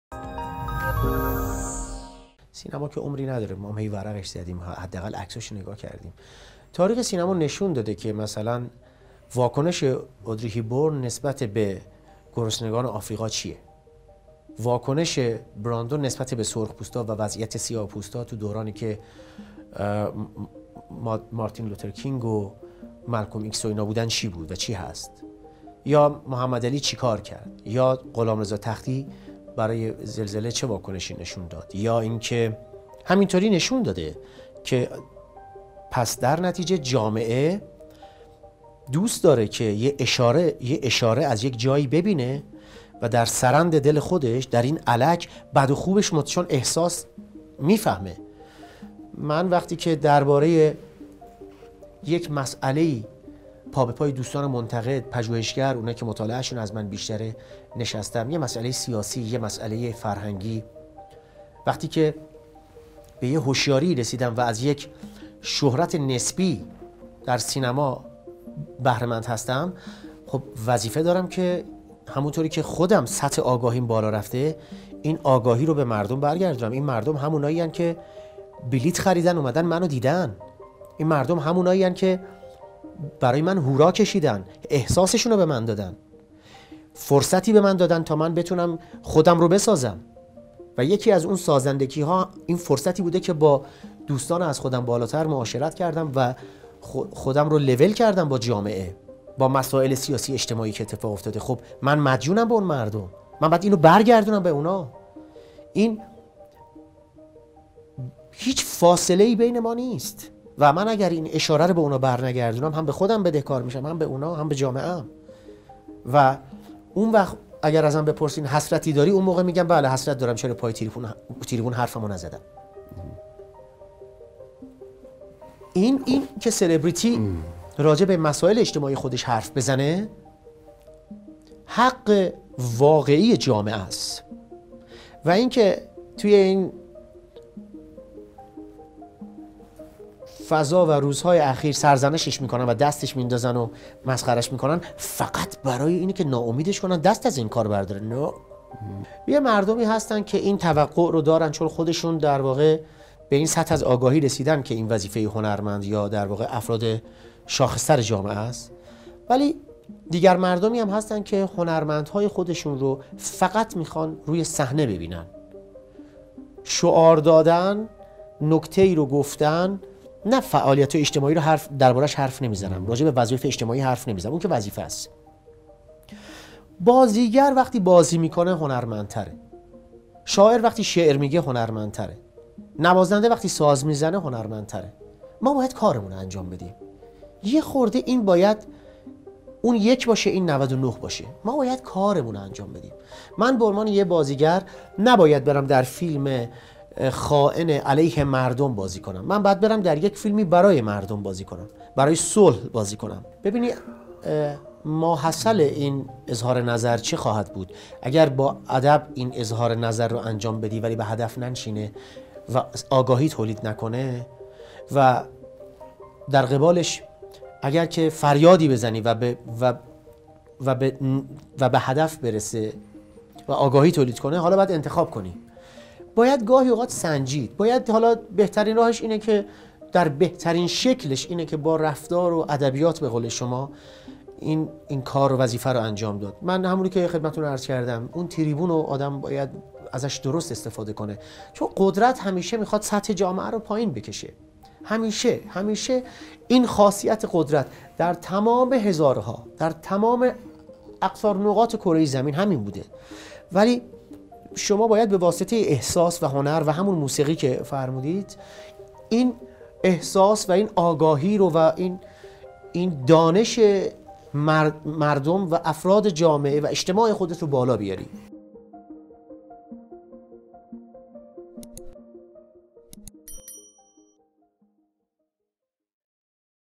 حامد بهداد | بخش هایی از مصاحبه | من در فیلمی که خائن به مردم است بازی نمی کنم
رضا عطاران | بخش هایی از مصاحبه | من در فیلمی که خائن به مردم است بازی نمی کنم

مصاحبه کامل حامد بهداد را در صفحه دیدآرت ببینید
به لیست مصاحبه های اختصاصی ما سر بزنبد